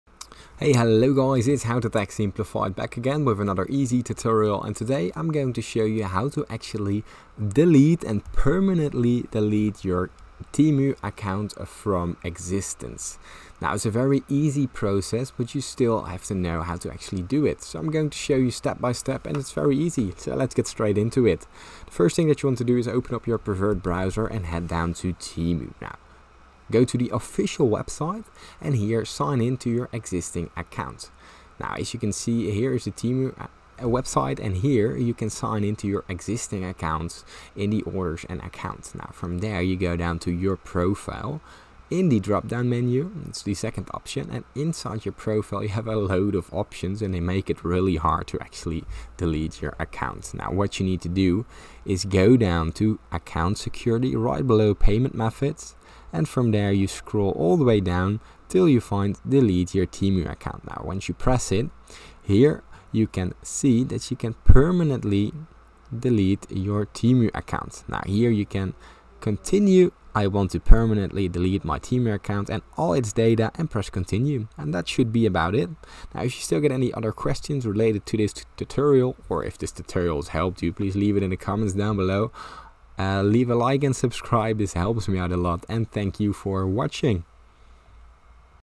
How To Delete TEMU Account - Permanently Delete TEMU Account
Say goodbye to your TEMU account with our step-by-step guide on how to delete it permanently. In this tutorial, we cover everything you need to know about closing your TEMU account, ensuring a smooth process from start to finish. Whether you're searching for 'How to delete TEMU account,' 'Permanently delete TEMU account,' or 'TEMU account closure,' this video has you covered. We navigate through the account settings, provide clear instructions, and address common questions to make the process hassle-free. Save time and watch now for a stress-free account deletion experience.

Also check out the following TEMU videos!